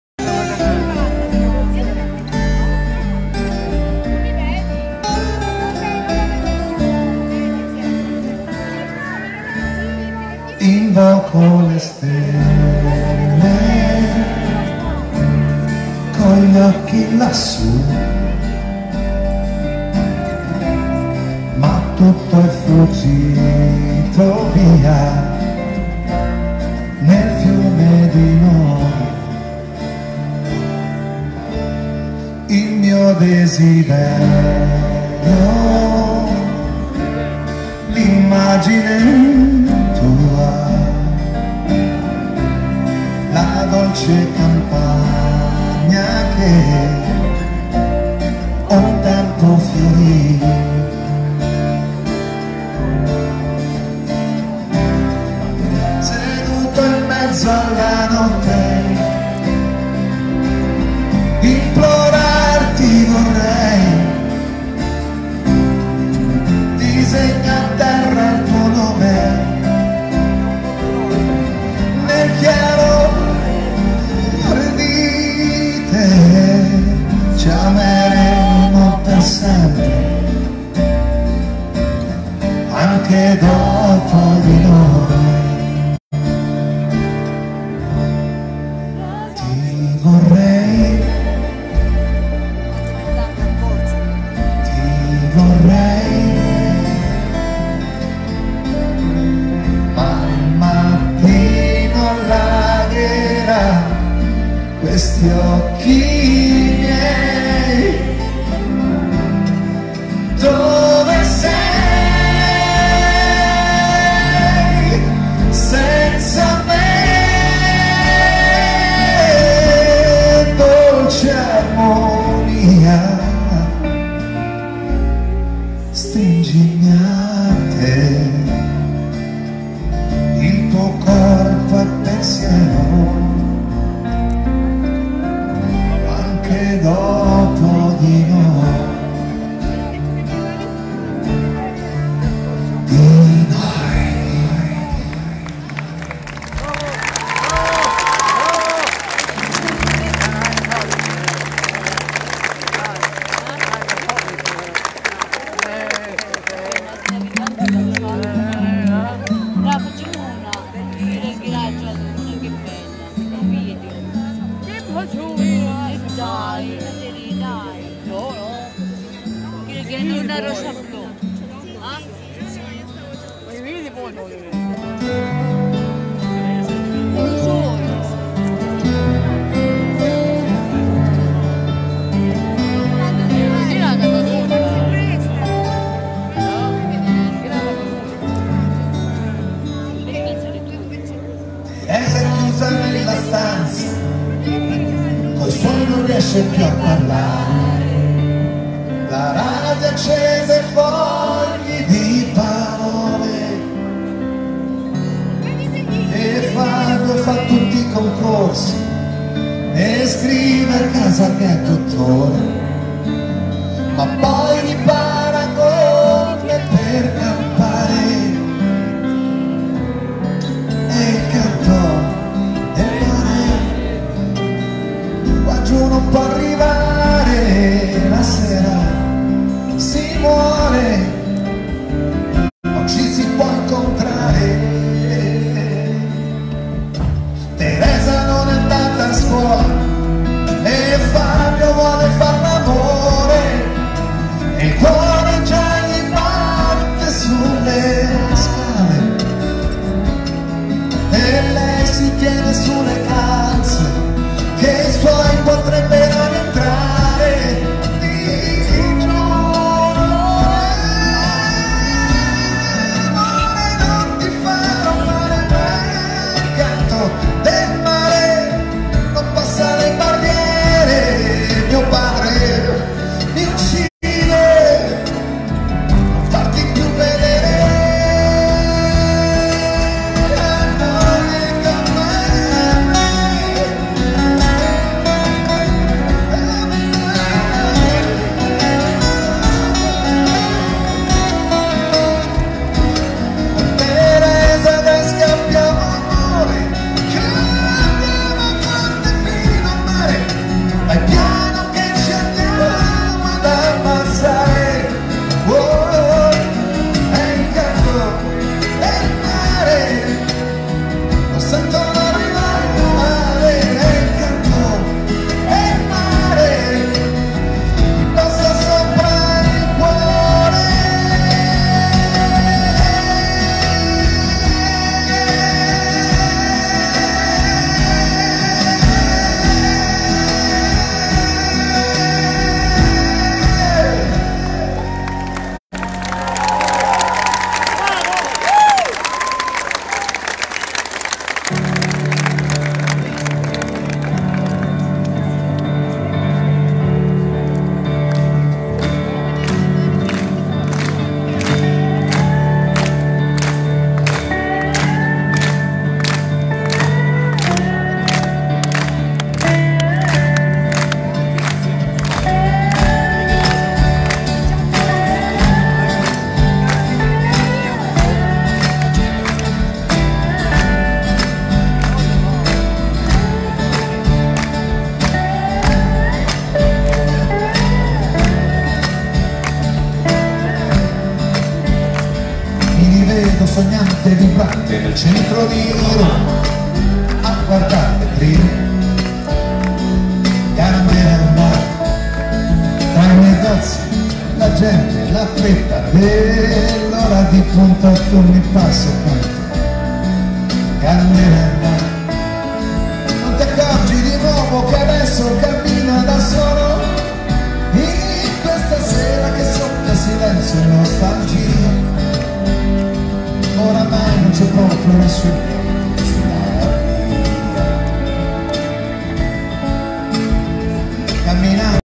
Concerto di Michele Zarrillo a Taurianova. (09/09/2018).
Stralcio del bellissimo concerto tenuto a Taurianova in occasione dei festeggiamenti di Maria SS della Montagna.